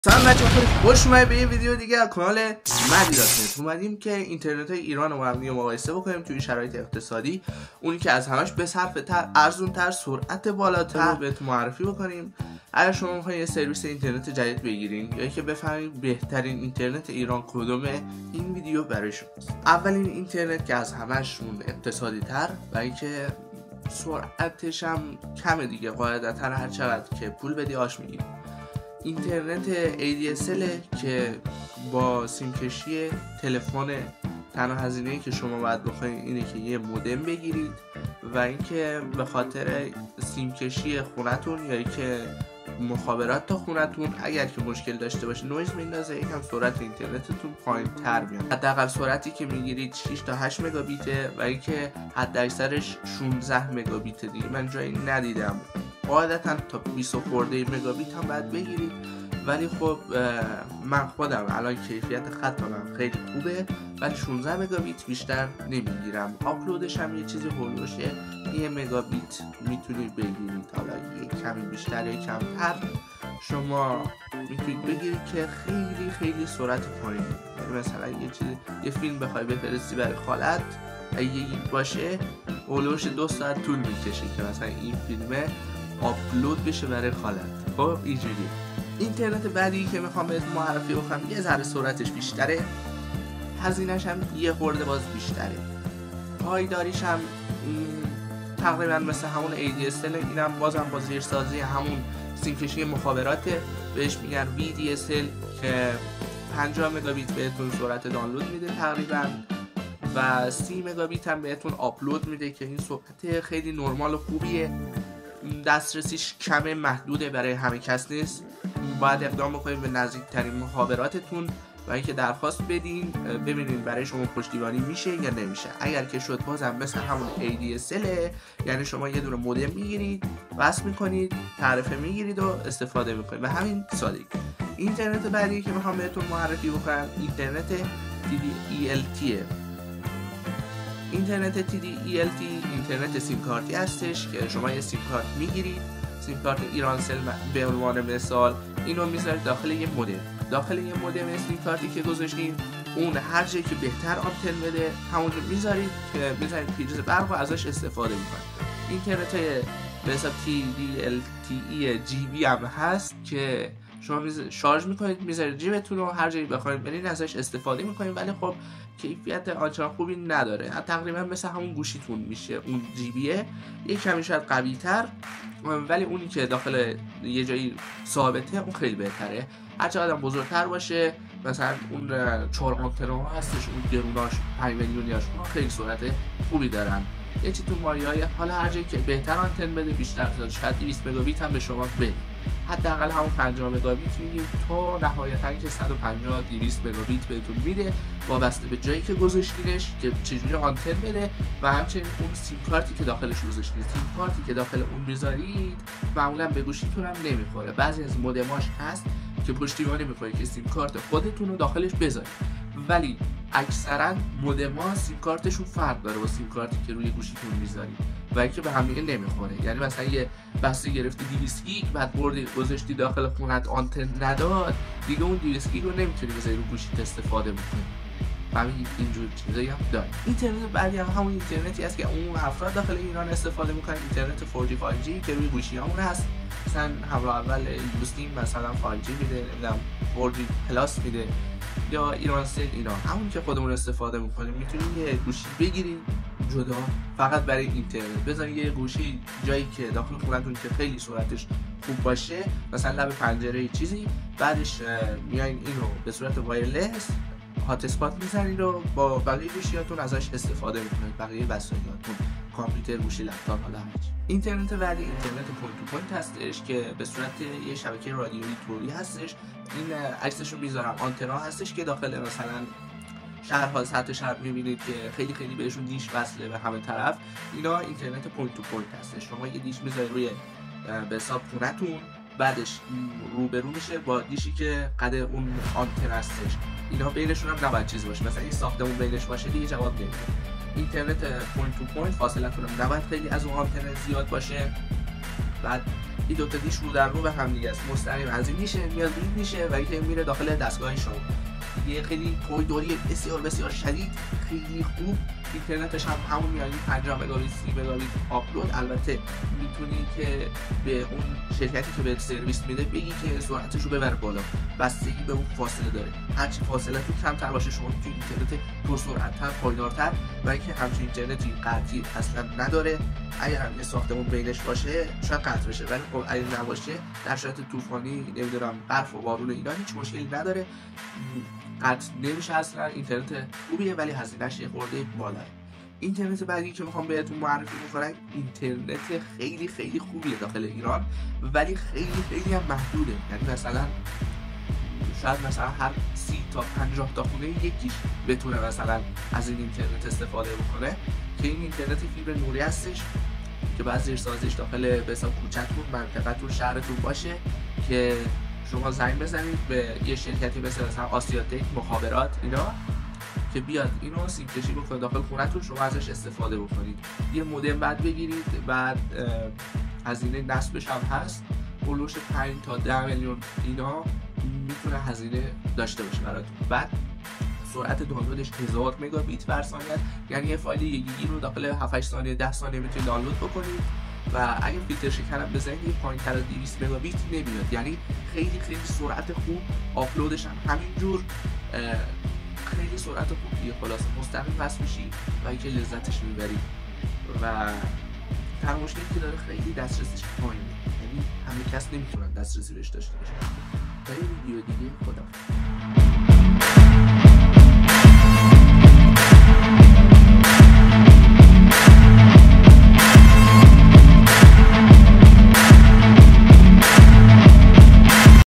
بهترین اینترنت ایران چیه/سریع ترین اینترنت/مقایسه اینترنت ایران/ پرسرعت ترین اینترنت خانگی Mahdi.Net
Mahdi.Net
در این ویدیو در این ویدیو می فهمیم پر سرعت ترین اینترنت ایران چیه و با مقایسه اینترنت های ایران میفهمیم بهترین اینترنت ایران چیه
:در این ویدیو به جواب سوال هایی مثل
1_بهترین اینترنت ایران چیه؟
2_سریع ترین اینترنت چیه؟
3_بهترین اینترنت خانگی چیه؟
4_از کجا اینترنت بگیریم؟
میرسیم .
00:00  مقدمه
00:19 ADSL ای دی اس ال
01:28 VDSL وی دی اس ال
02:14 TD-ELT تی دی ال تی
03:50 4G 5G
04:24 Point-to-point پوینت تو پوینت
05:36  Optical fiber فیبر نوری
((هر سوالی دارید در کامنت ها بگید ایده برای ویدیو بعد بگین خیلی سریع پاسخ میدم.)) ساب کردن یادتون نره
به اینا سر بزن  !
اگر حتی چیز کوچکی یاد گرفتی لایک کن.